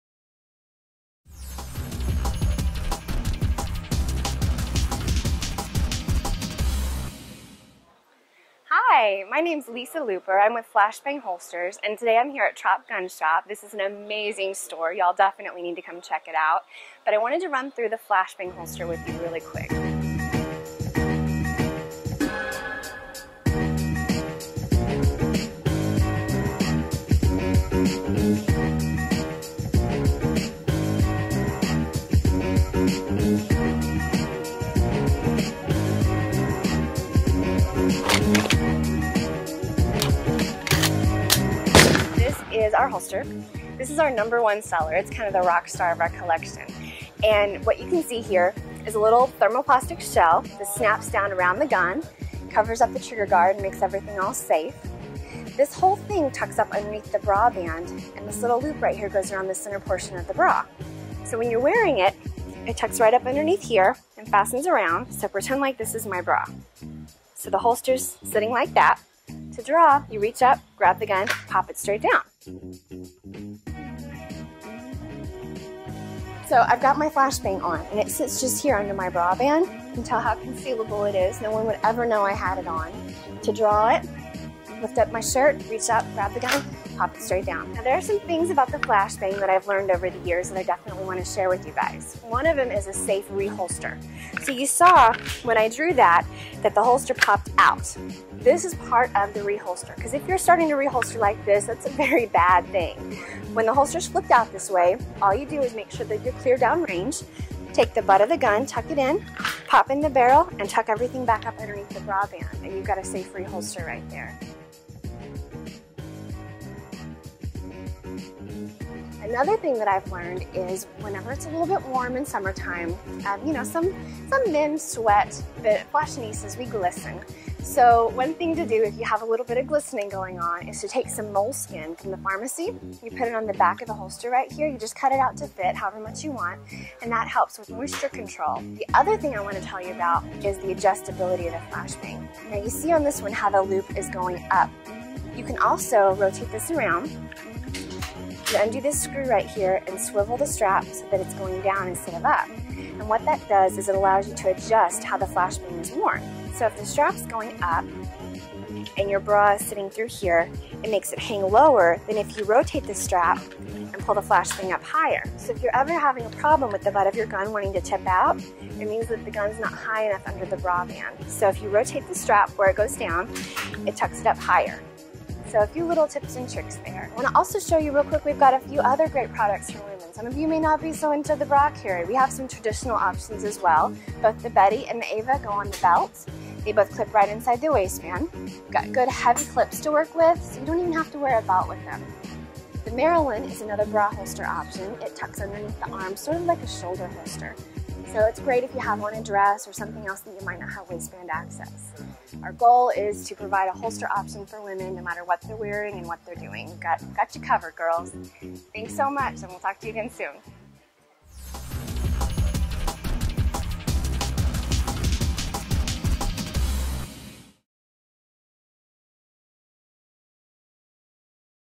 Flashbang demo
Lisa Looper shows some Flashbang bra holsters options.